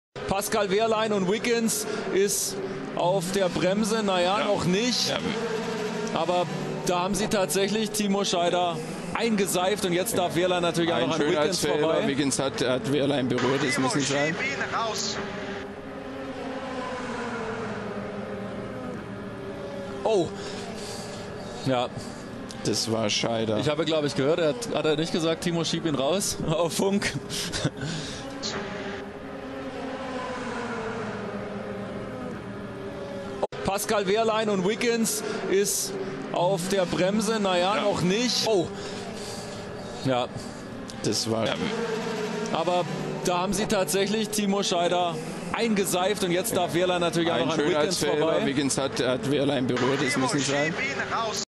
DTM 2015 Spielberg Race 2 ARD Timo Scheider Team Radio
Push him off! Scheider pushes off Wehrlein and Wickens to the command of his team.

Watch the second DTM race at Spielberg from the unique perspective of Audi racer Timo Scheider. Two onboard cameras and pit radio throughout the race .

Watch race 2 at Spielberg from the perspective of Timo Scheider (Audi RS 5 DTM). Race 2: Rennen 2: .

Timo Scheider with a highly controversial move towards the end of race 2 at Spielberg, pushing both Wickens and, in turn, Wehrlein off the track. Timo Scheider .